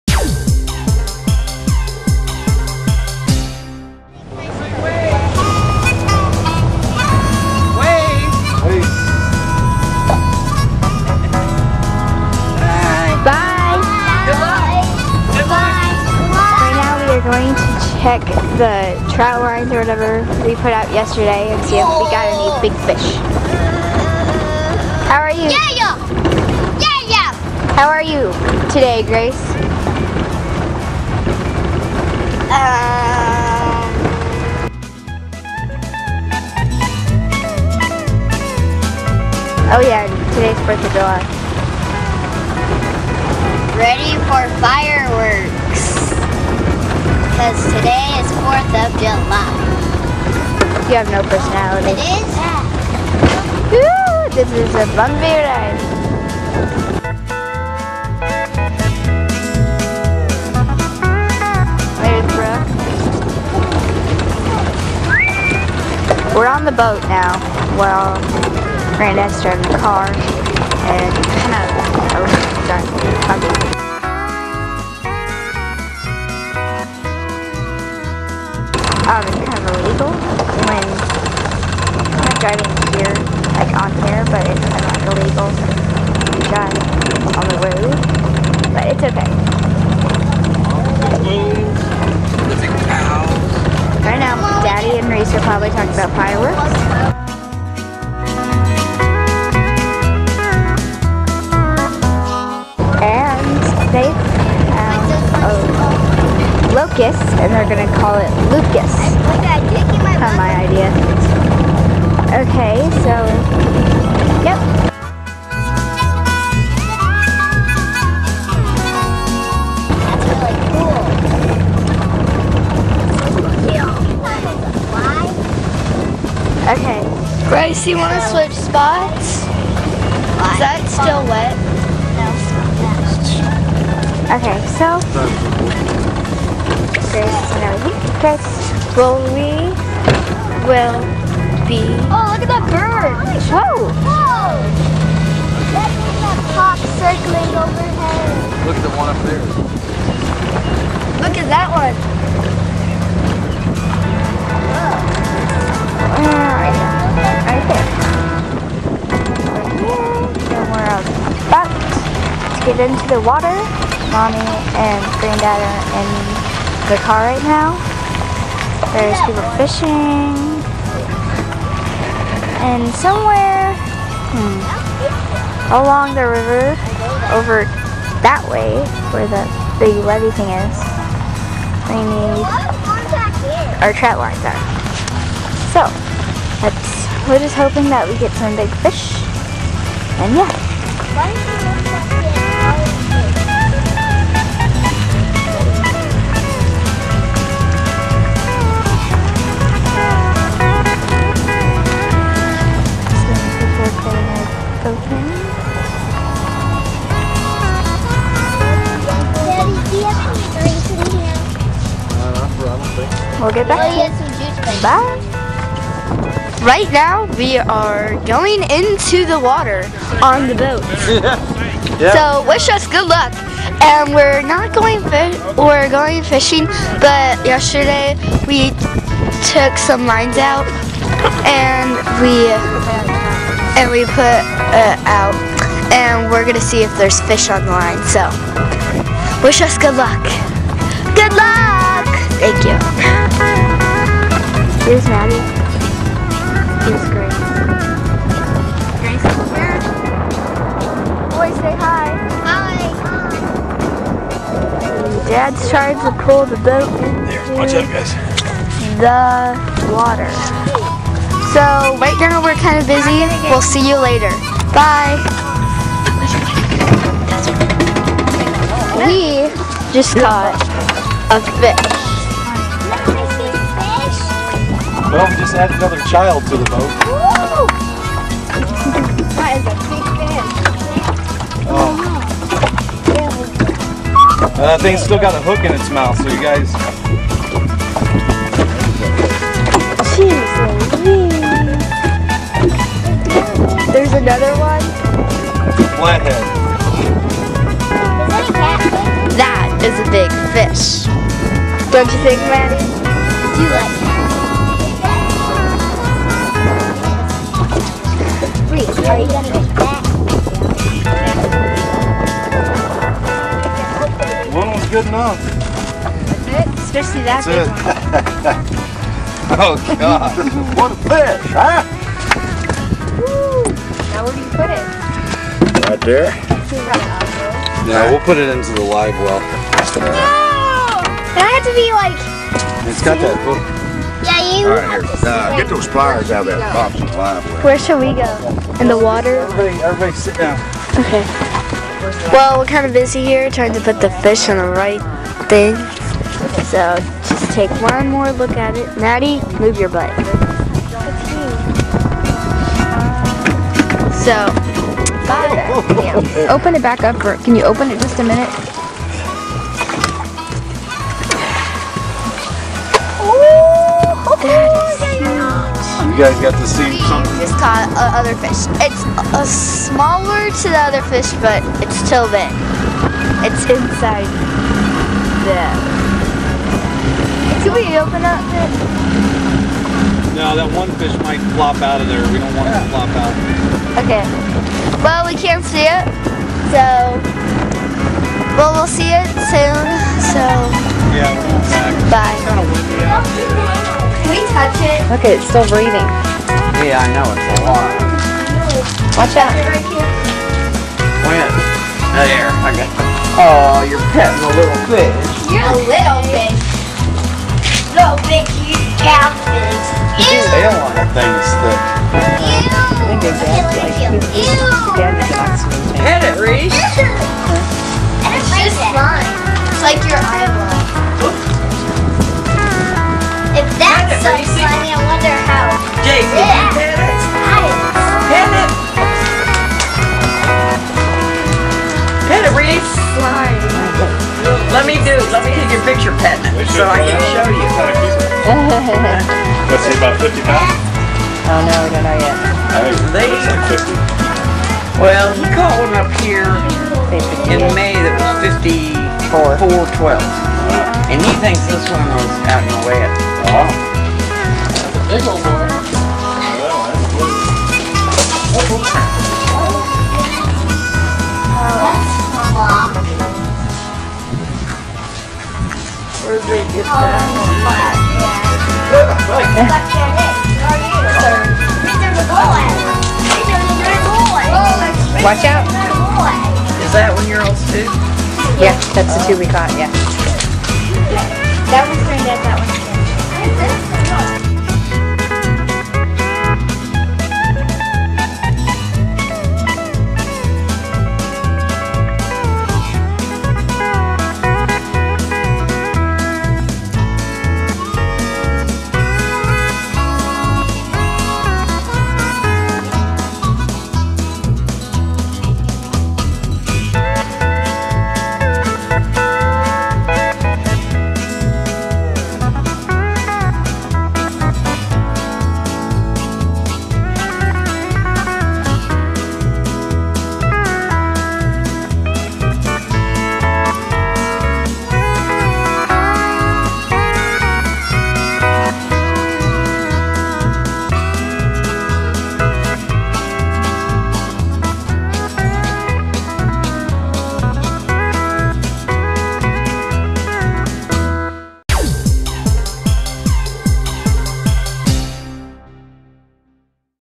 M.A.D. 21 Fishing Trip 2009
It's a hog! It's a hog! Watch Maddie and her family explore the adventures of fishing!